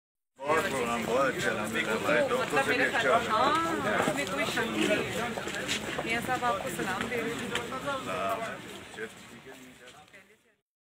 Maryam Nawaz arrived At Gilgit Airport
Maryam Nawaz arrived At Gilgit Airport safely and she was received by EX CM Hafeez ur Rehman